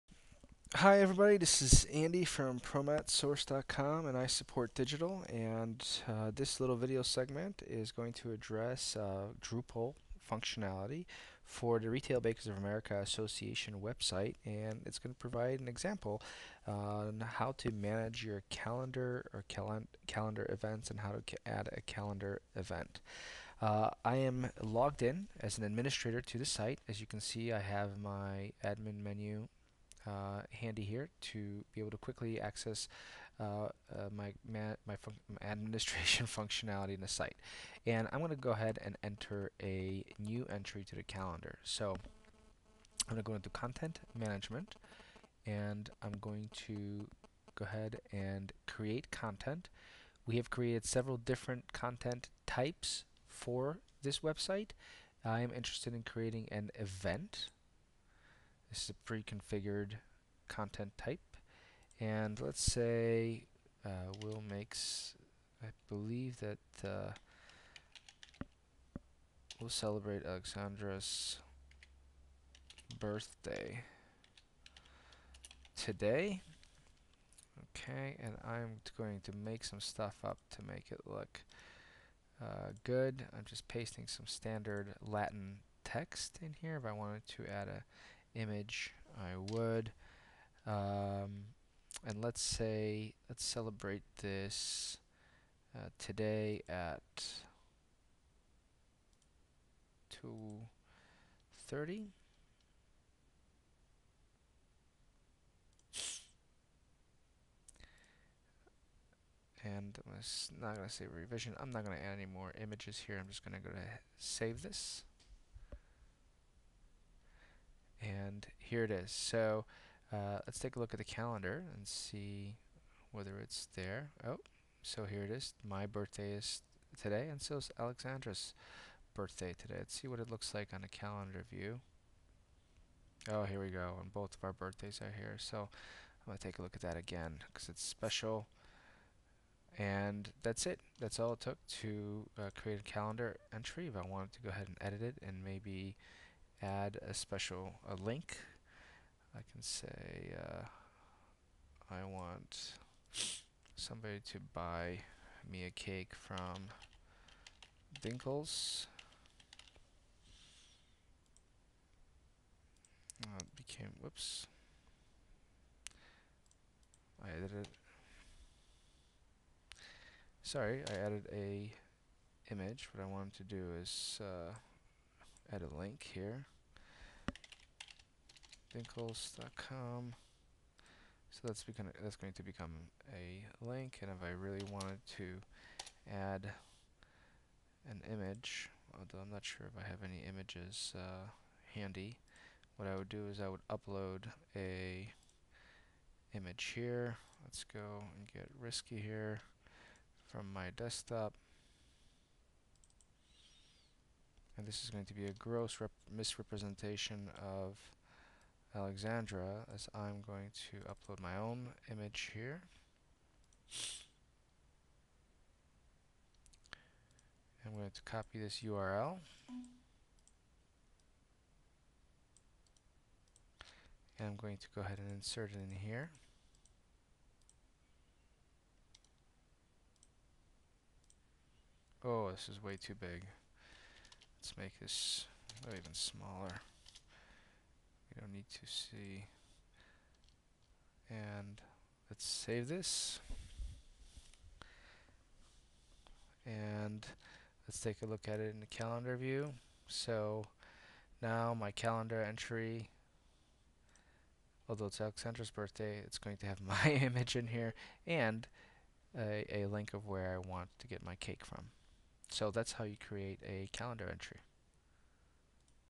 Drupal Administration - Editing Calendar entries example
Promet Source & IsupportDigital demonstrate drupal 6 content management: calendar entry creation.  drupal admin menu, event management, wysiwyg, image upload.